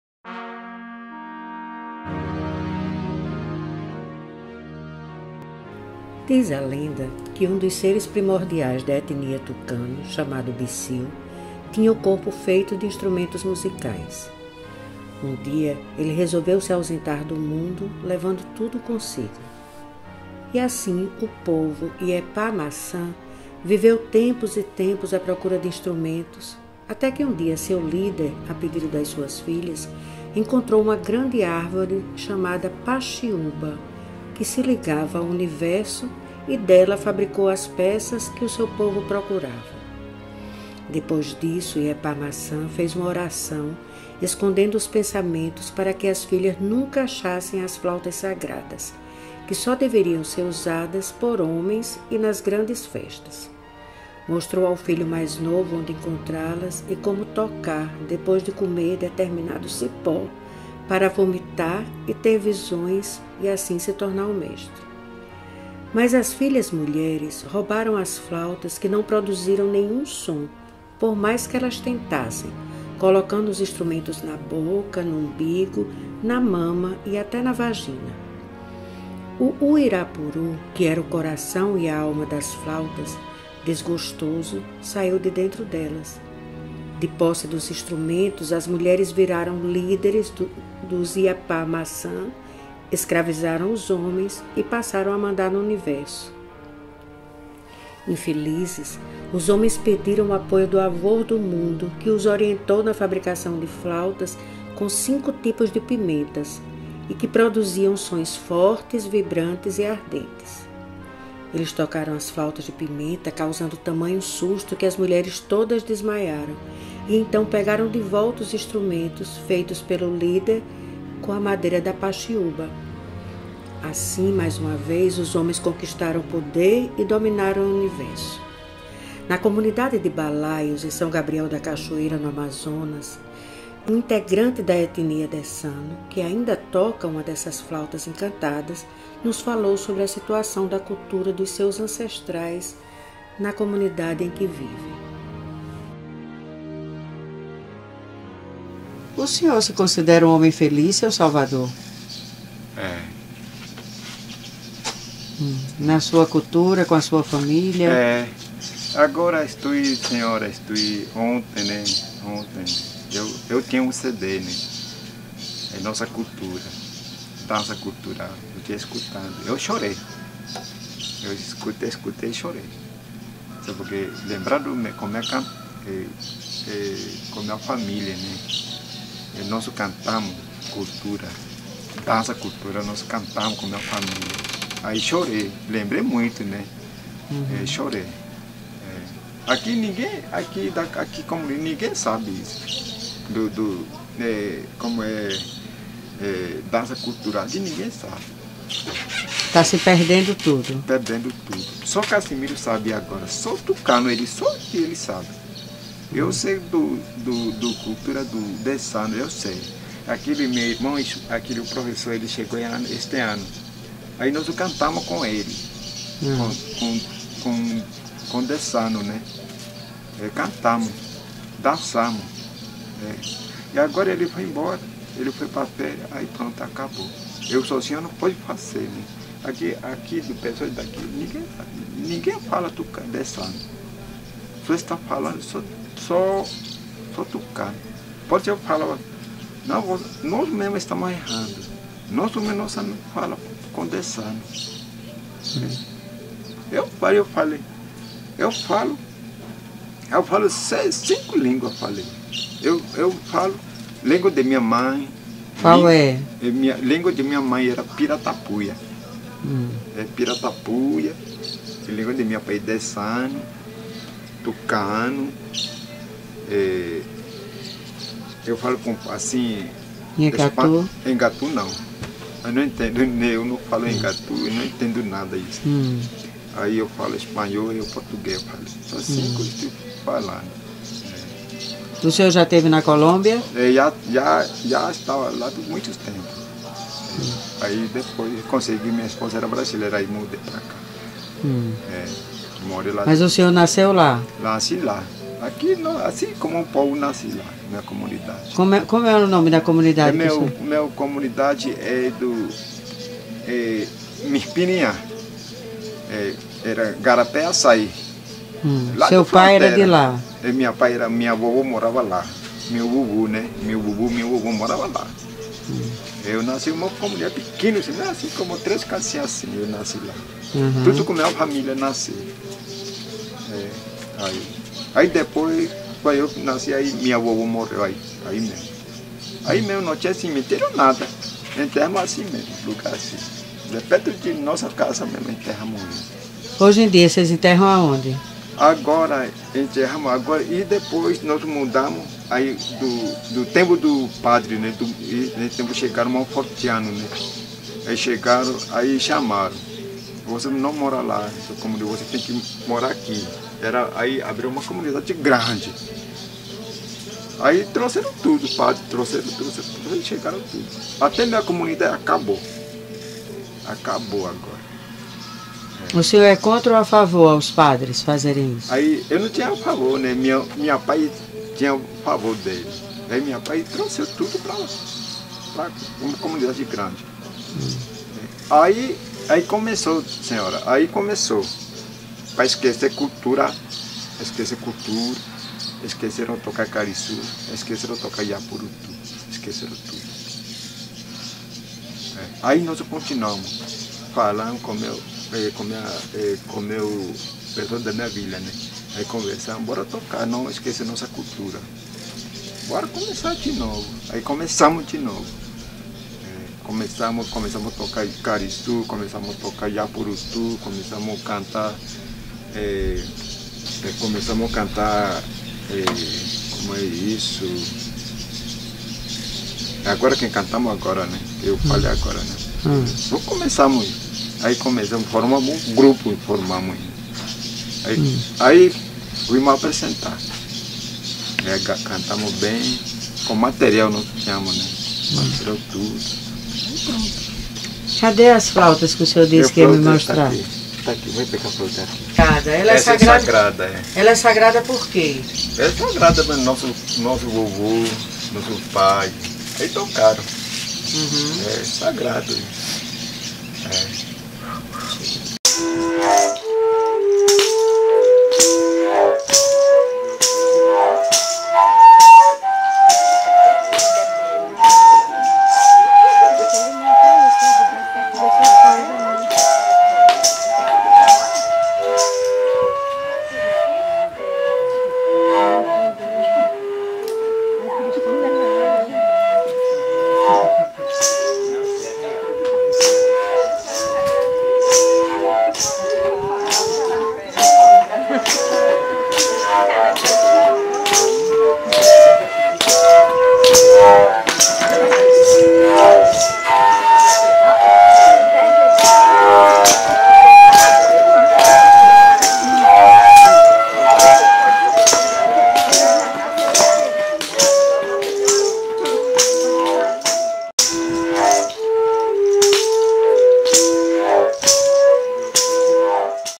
CULTURA DESANO
Diz a lenda que um dos seres primordiais da etnia tukano chamado BISIU tinha o corpo feito de instrumentos musicais.
Um dia ele resolveu se ausentar do mundo levando tudo consigo. Assim, o povo Yepá Mahsã viveu tempos e tempos a procura de instrumentos até que um dia seu líder, a pedido das suas filhas, encontrou uma grande árvore chamada PAXIÚBA que se ligava ao universo e dela fabricou as peças que o seu povo procurava.
Depois Yepá Mahsã fez uma oração escondendo os pensamentos para que as filhas nunca achassem as flautas sagradas que só deveriam ser usadas por homens e nas grandes festas.
Mostrou ao filho mais novo onde encontrá-las e como tocar depois de comer determinado cipó para vomitar, ter visões e se tornar um mestre.
Mas as filhas mulheres roubaram as flautas que não produziram nenhum som por mais que elas tentassem colocando os instrumentos na boca, no umbigo, na mama e até na vagina!
O UIRAPURU que era o coração e a alma das flautas, desgostoso, saiu de dentro delas. De posse dos instrumentos as mulheres viraram líderes dos Yepá Mahsã, escravizaram os homens e passaram a mandar no universo.
Infelizes, os homens pediram o apoio do Avô do Universo que os orientou na fabricação de flautas com 5 tipos de pimentas e que produziam sons fortes, vibrantes e ardentes. Eles tocaram as flautas de pimenta causando tamanho susto que as mulheres todas desmaiaram. Então, pegaram de volta os instrumentos feitos pelo líder com a PAXIÚBA.
Assim, mais uma vez os homens conquistaram o poder e dominaram o universo.
Encontramos na Comunidade Balaios, na Amazônia, um integrante da etnia desano que toca uma dessas flautas encantadas. Ele falou sobre a situação da cultura dos seus ancestrais na comunidade em que vive.
Helenita Monte de Hollanda